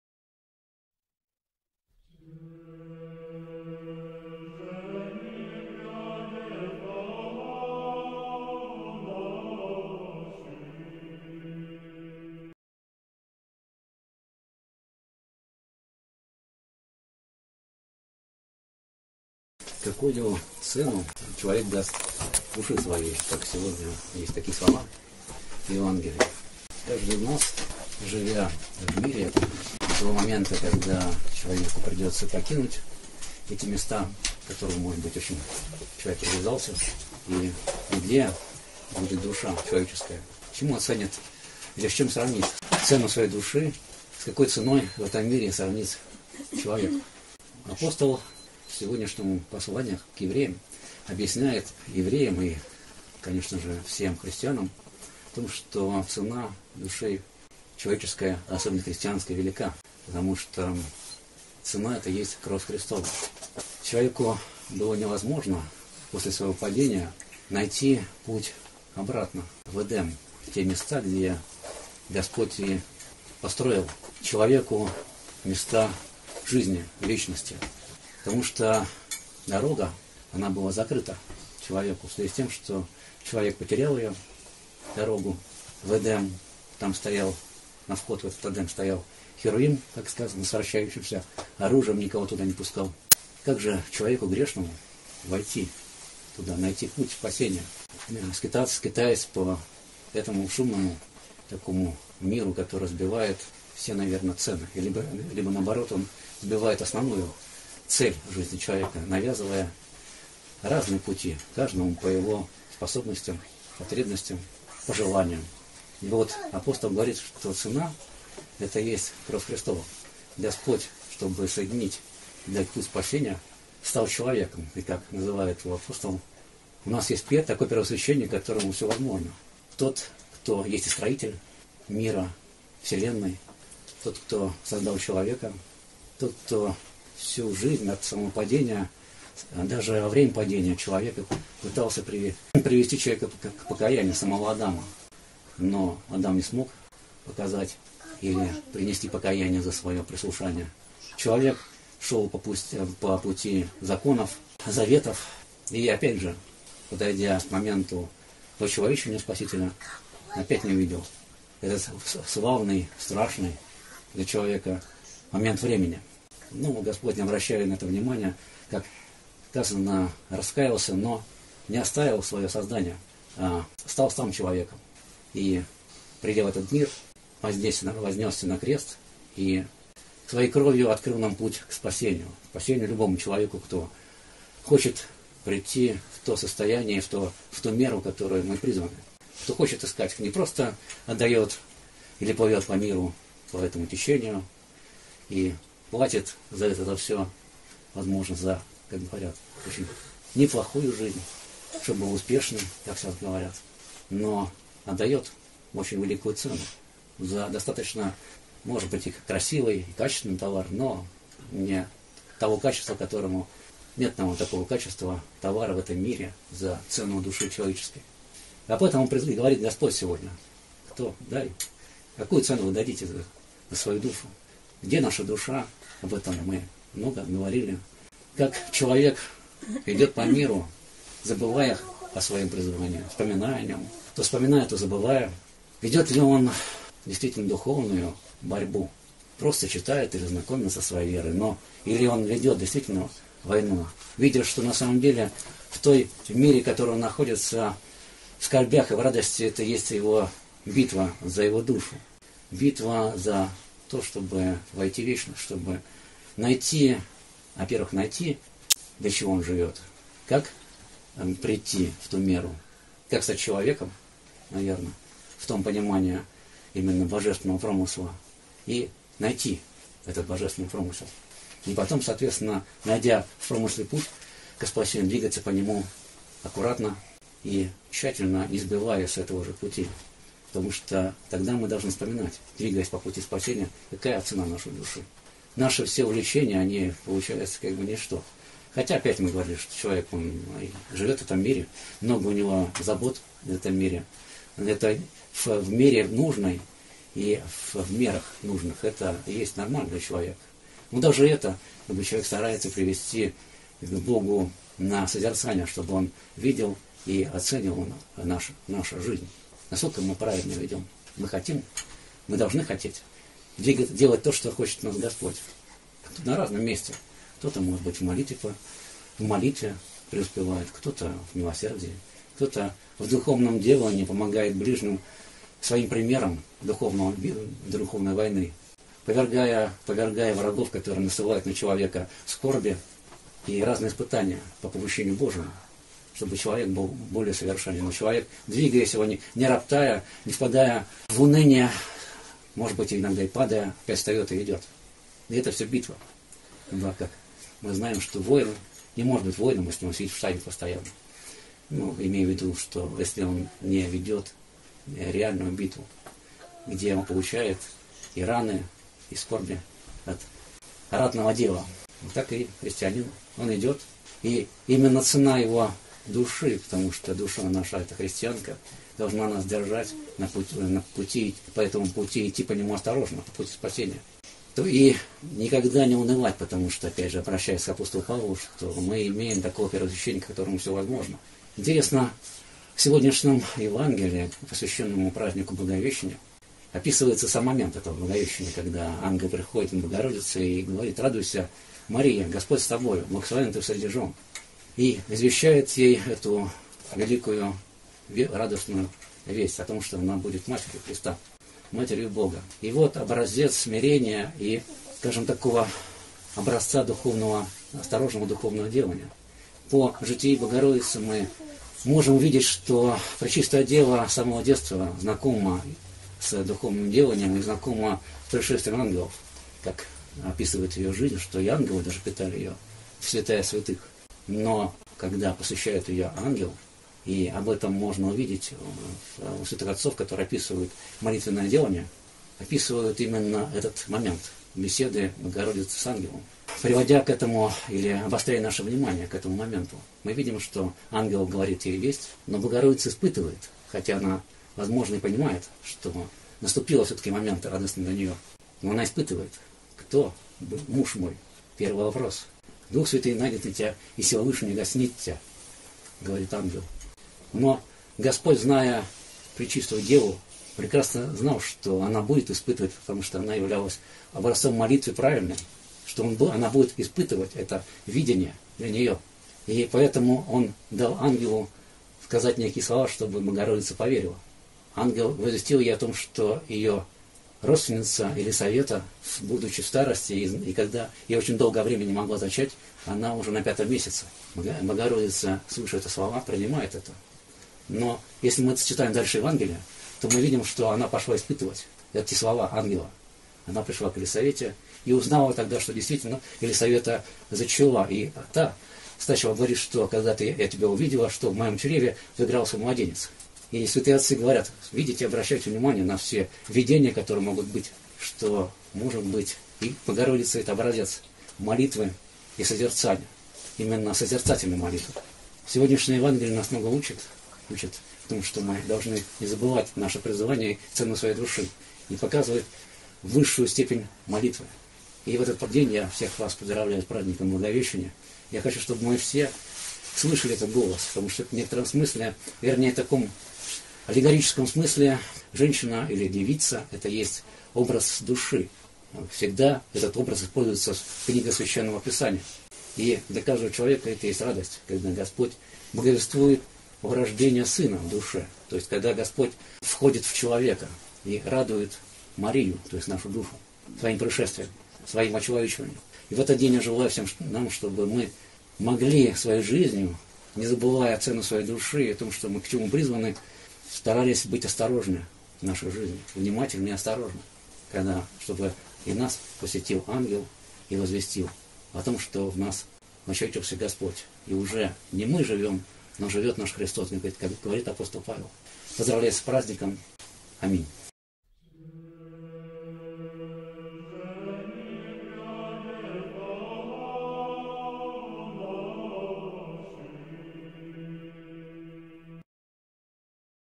Цена души - Кровь Христова
Благовещение Пресвятой Богородицы
Проповедь Епископа Афанасия, правящего архиерея Центрально-Российской Епархии
Поздравив молящихся с участием в Божественной Литургии и Таинстве Евхаристии, епископ Афанасий (Жюгжда), в своей проповеди сказал:
«… Какую цену даст человек душе своей?.. Каждый из нас, живя в мире, до того момента, когда придется покинуть эти места, … и где будет душа человеческая?.. С чем сравнить цену своей души? С какой ценой в этом мире сравнить человека? … Цена души человеческой – Кровь Христова!..»
В оформлении: Благовещение, диптих (1898). Нестеров Михаил Васильевич.
Россiйская Православная Церковь
2024г. от Р.Х.